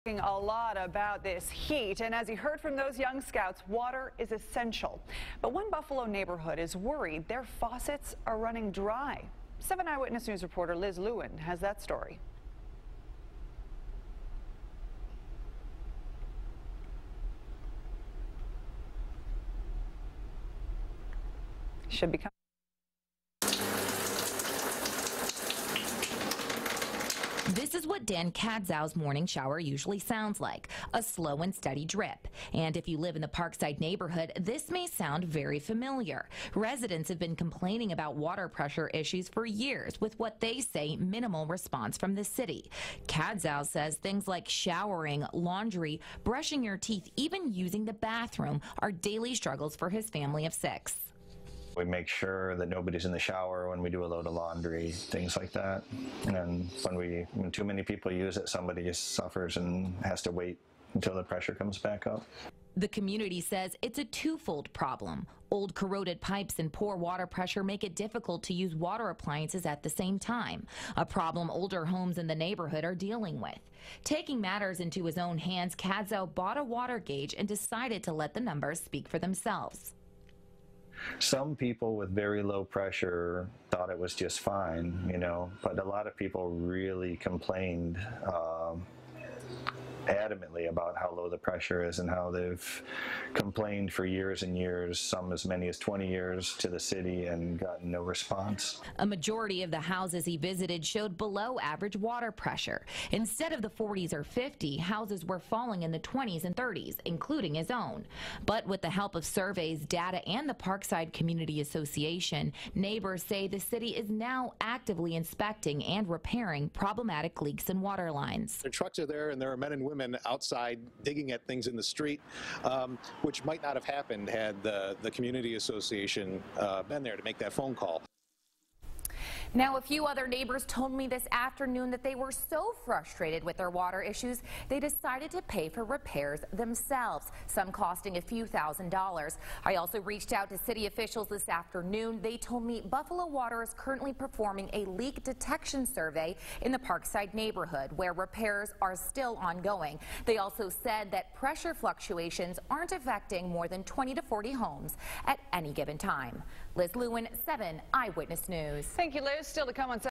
Parkside Community Speaks Out On Water Pressure
After dealing with fluctuating water pressure in their neighborhood for years, residents in the Parkside neighborhood are now speaking out and taking action - together. With the support of the Parkside Community Association, residents hope city officials will quickly and effectively address the problem.

7 Eyewitness News is Buffalo's Source for news, sports, weather, and good things happening every day.